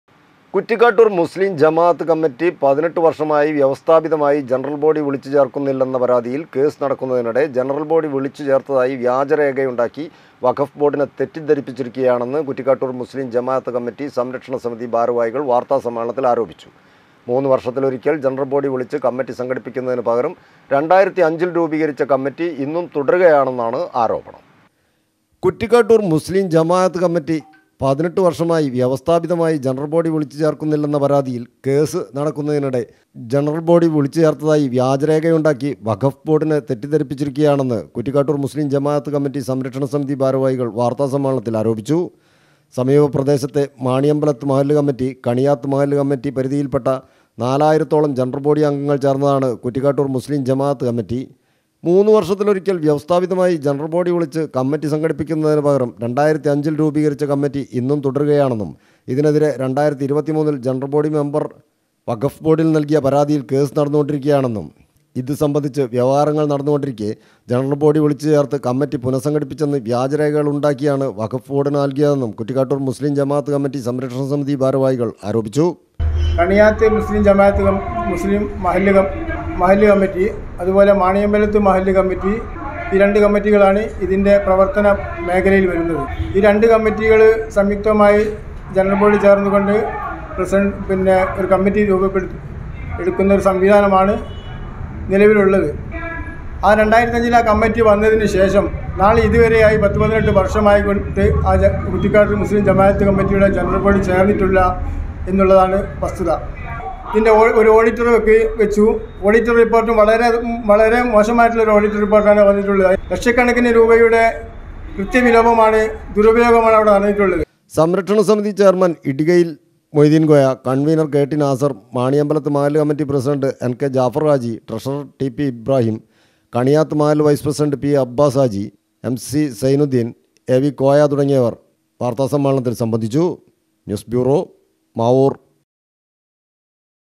ജമാഅത്ത് കമ്മിറ്റി 18 വർഷമായി വ്യവസ്ഥാപിതമായി ജനറൽബോഡി വിളിച്ചു ചേർക്കുന്നില്ലെന്ന പരാതി
കുറ്റിക്കാട്ടൂർ മുസ്‌ലിം ജമാഅത്ത് കമ്മിറ്റി 18 വർഷമായി  വ്യവസ്ഥാപിതമായി ജനറൽബോഡി വിളിച്ചു ചേർക്കുന്നില്ലെന്ന  പരാതിയിൽ കേസ് നടക്കുന്നതിനിടെ ജനറൽ ബോഡി വിളിച്ചു ചേർത്തതായി വ്യാജരേഖയുണ്ടാക്കി വഖഫ് ബോഡിനെ തെറ്റിദ്ധരിപ്പിച്ചിരിക്കുകയാണെന്ന്  കുറ്റിക്കാട്ടൂർ മുസ്‌ലിം ജമാഅത്ത് കമ്മിറ്റി സംരക്ഷണ സമിതി